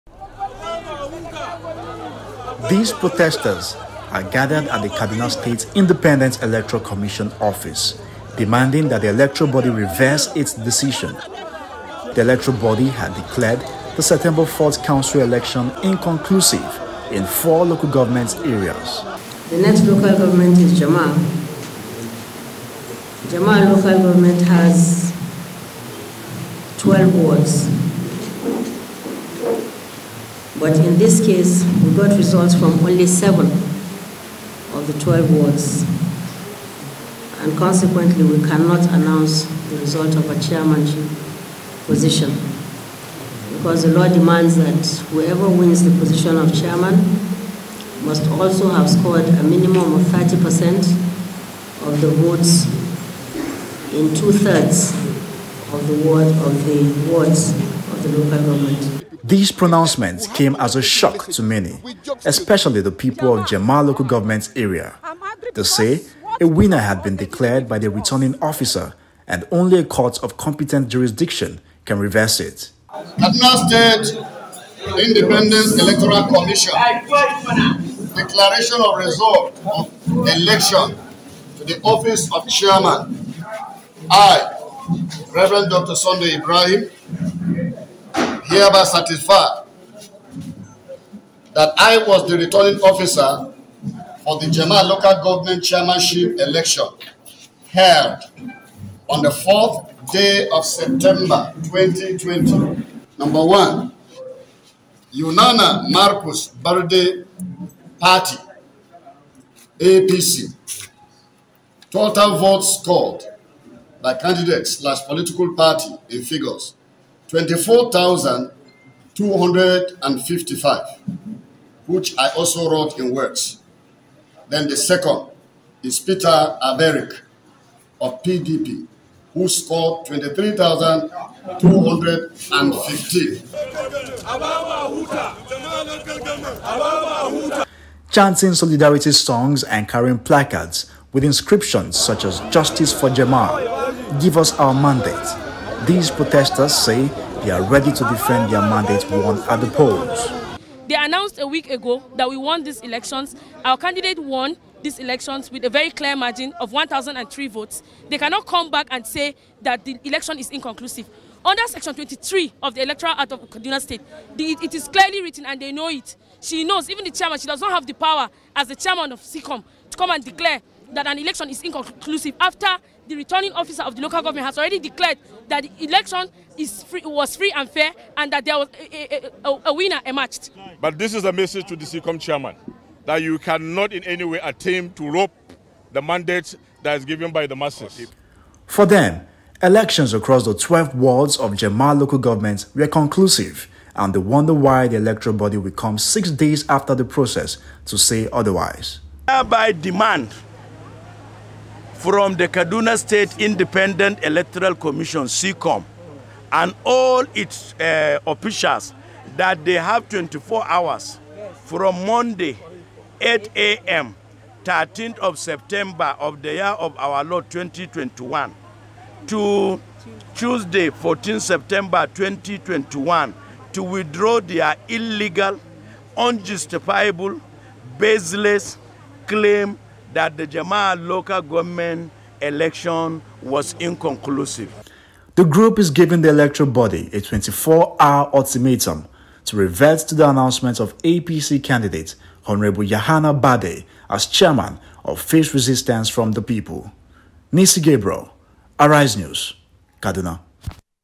PROTEST IN KADUNA OVER LG POLLS - ARISE NEWS REPORT
Supporters of the All progressives congress have rejected the declaration of the council election in Jema'a local government area as inconclusive by the Kaduna State electoral commission. They want the electoral body to reverse itself and declare the APC candidate the winner within 24 hours. Arise Correspondent Nissi Gabriel reports.